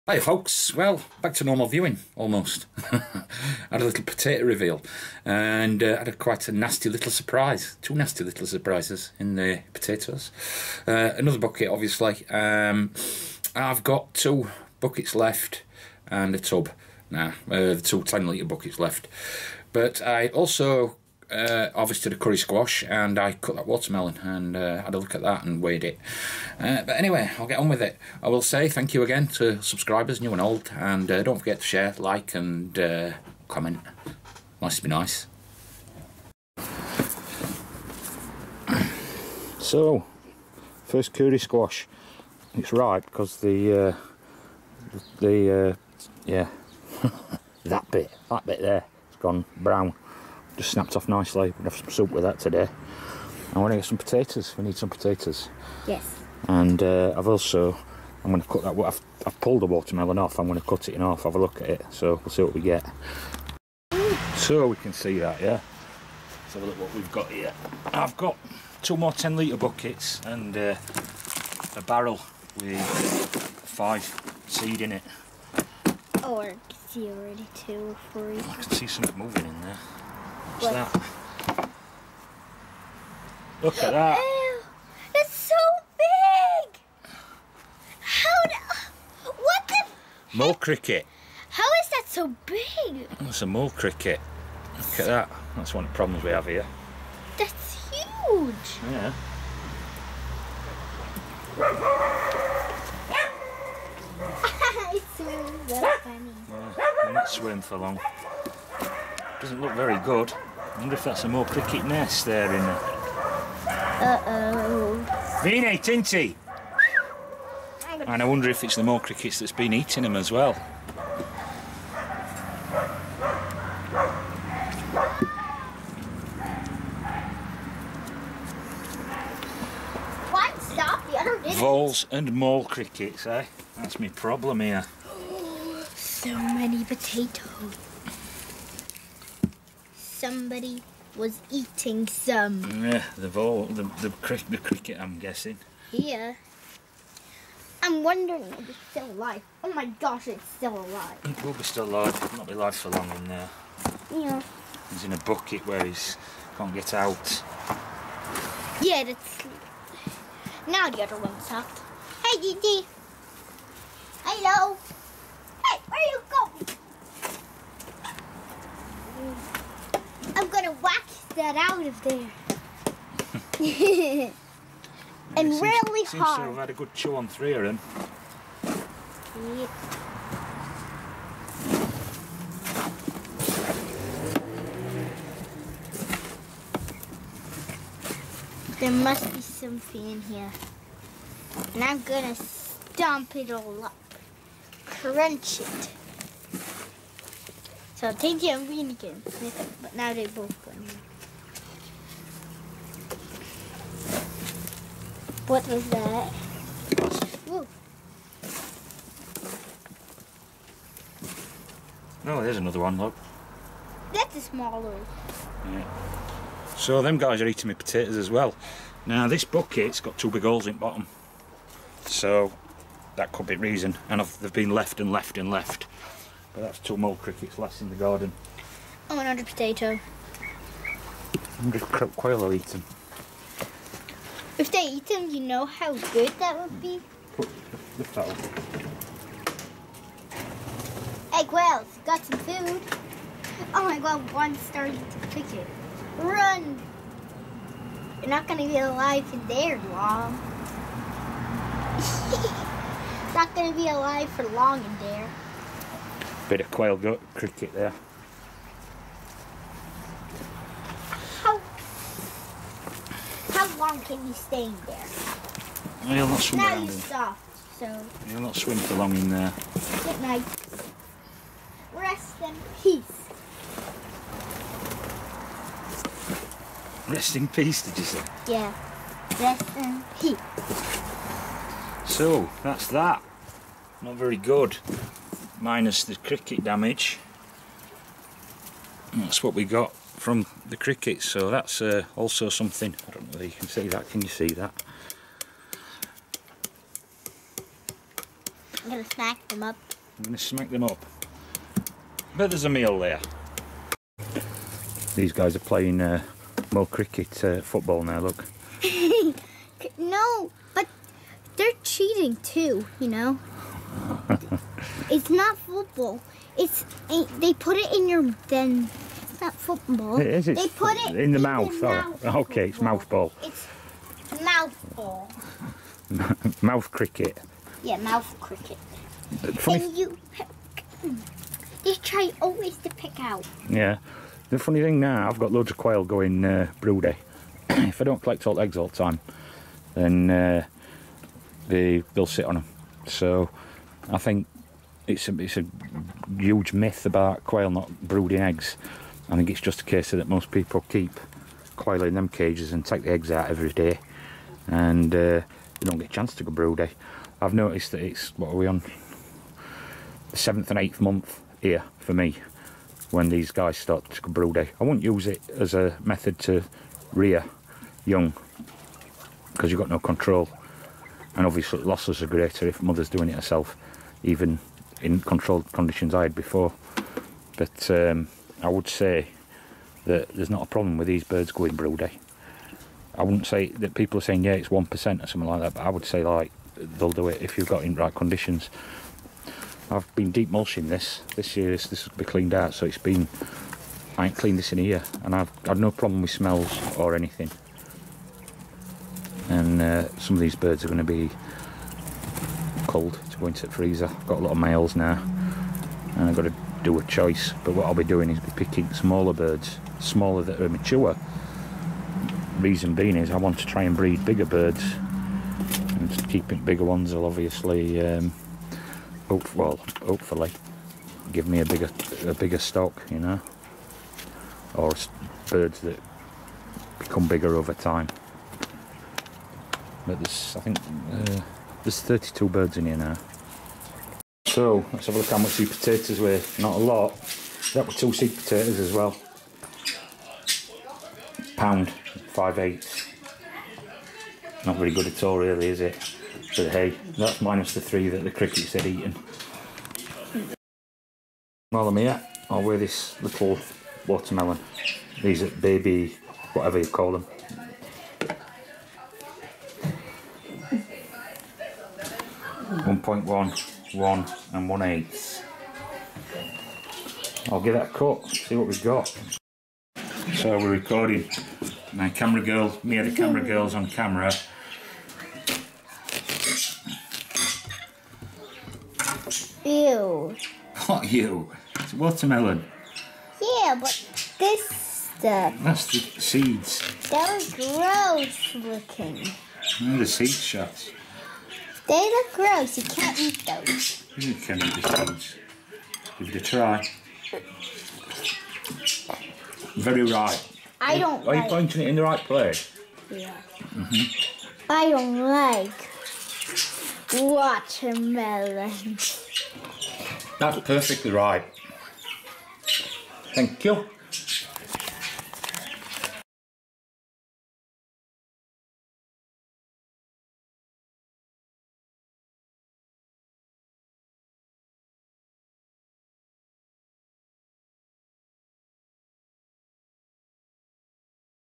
Kuri squash water melon and potato reveal 26 08 18
My melon was ripe ;-) we also needed some potatoes and a squash.
Kuri squash water melon and potato reveal